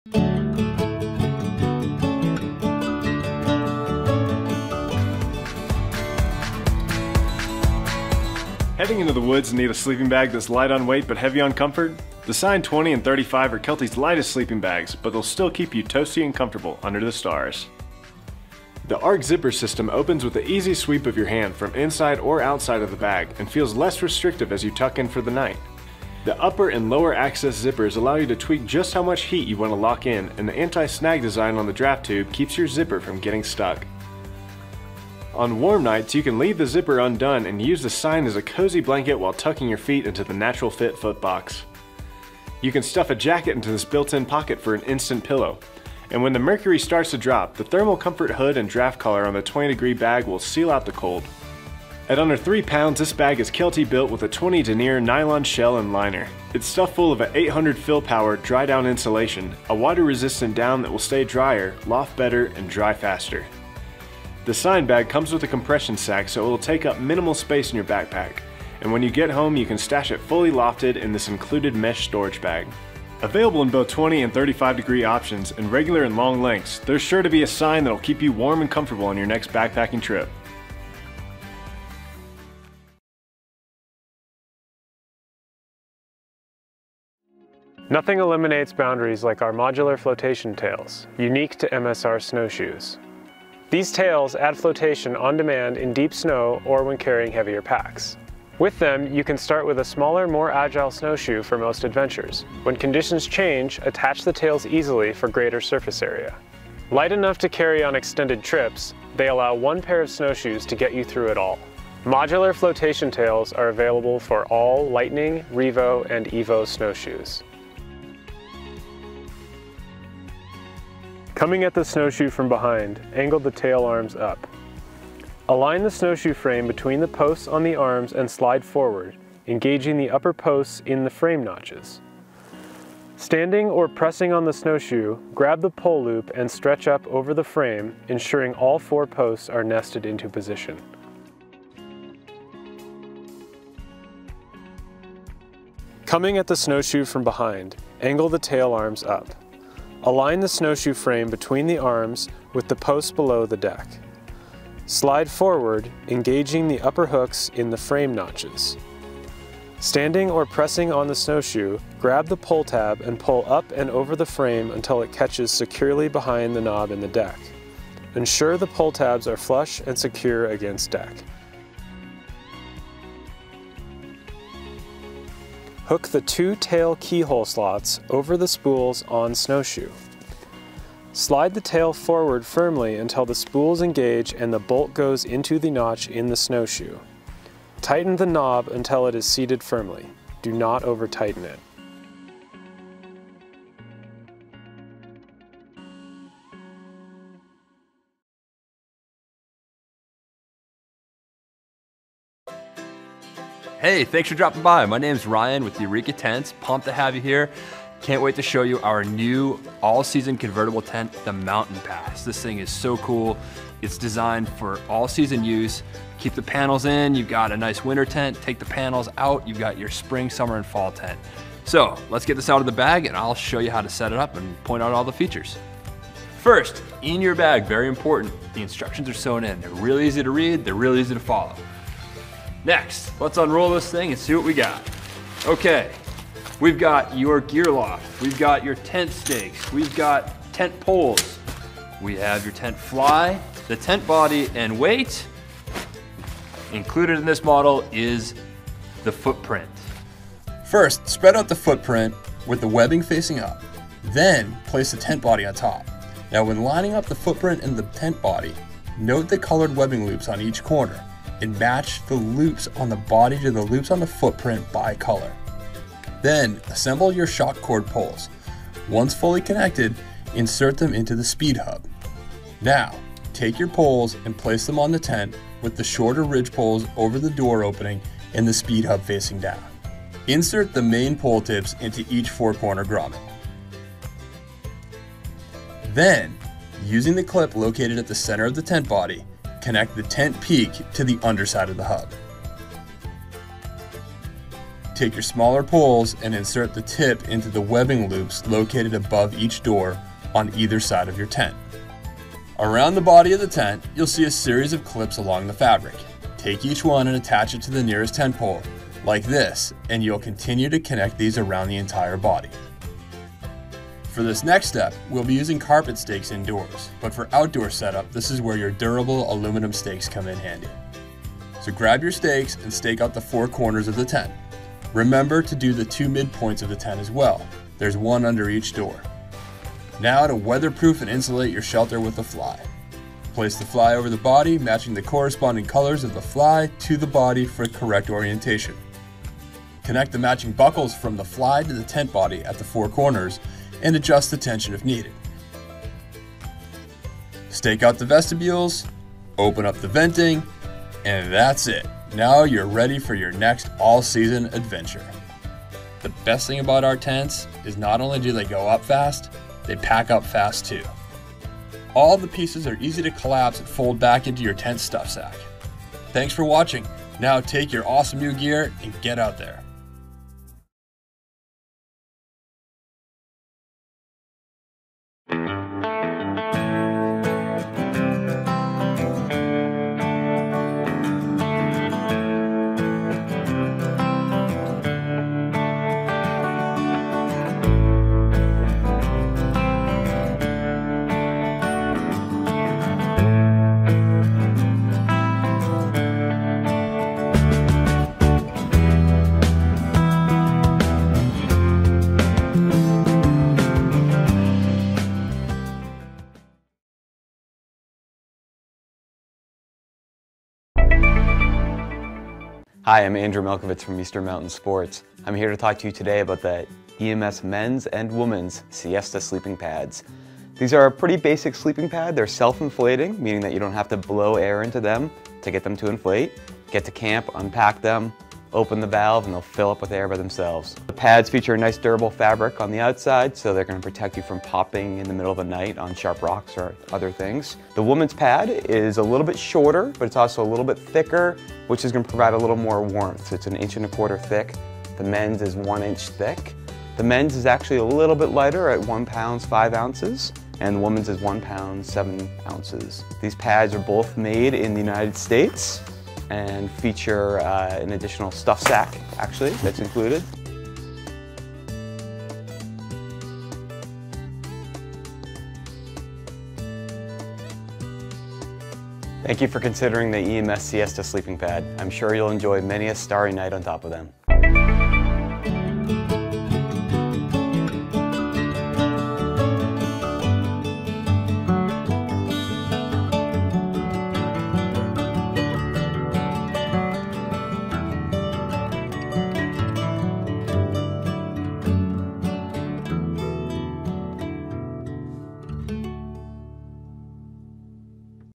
Top Quality Gear for Camping, Backpacking and Hiking & More #2
Visit the Latest Greatest store for great shirts to wear while out camping, hiking, or backpacking.

Links to the products shown in the video:

Kelty Sine DriDown Sleeping Bag:
at Amazon
MSR Snowshoe Flotation Tails:
at Amazon
Eureka Mountain Pass 2 Backpacking Tent
Marmot Featherless:
at Amazon
EMS Siesta Sleeping Pad:
at Amazon

You keep Latest Greatest online when you shop on Amazon using my links!